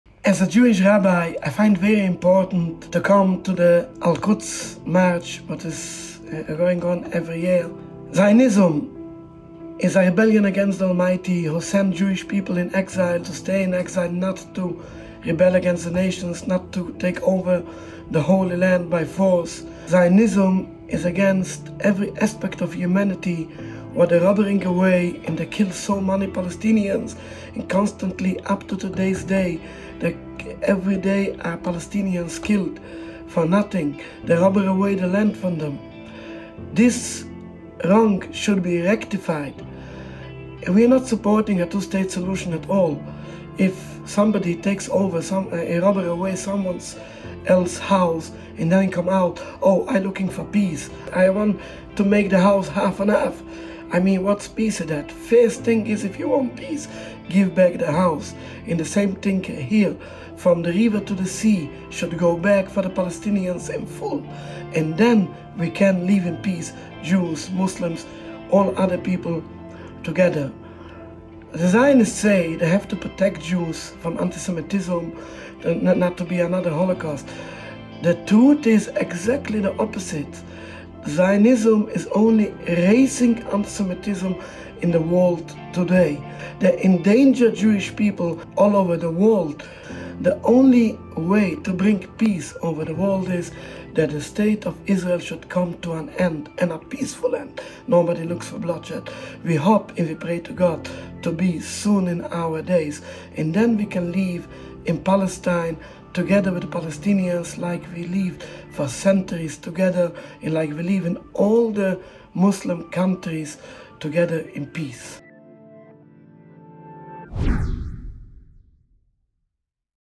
Anti-Zionist Jews support Palestine
As a Jewish Rabbi I find it very important to attend the Al-Quds rally every year. Here’s why,

Watch Rabbi Elhanan Beck’s important message for Palestine.